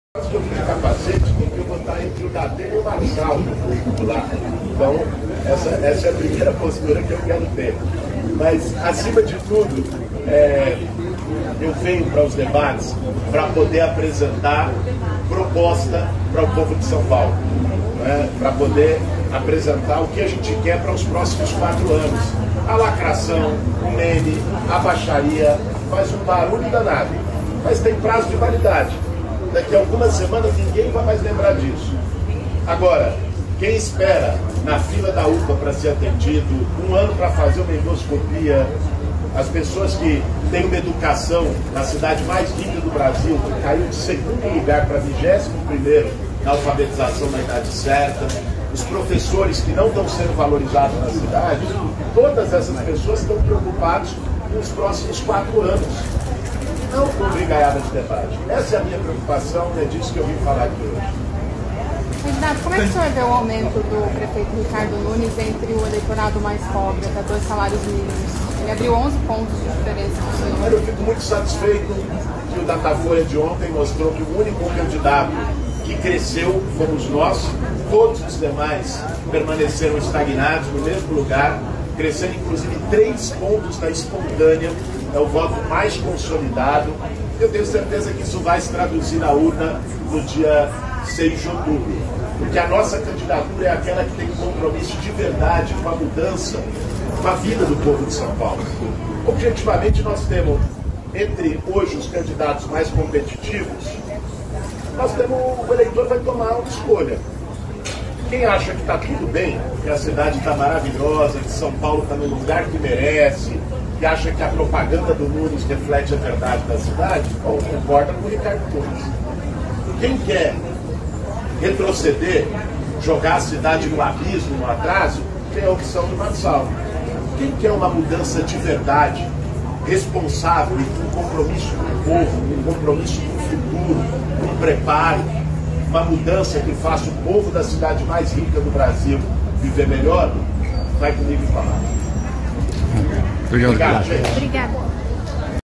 Guilherme Boulos diz que vai a debate para apresentar propostas
Guilherme Boulos afirmou que vai a debate, nesta sexta-feira (20/9), debater propostas. “Lacração, meme, baixaria fazem um barulho danado, mas têm prazo de validade”, criticou.

Os candidatos à Prefeitura de São Paulo nas eleições 2024 participam de um debate com a transmissão do Terra, em parceria com SBT e rádio Nova Brasil. A apresentação e mediação é do jornalista César Filho.

📸  Bruno Ribeiro/Metrópoles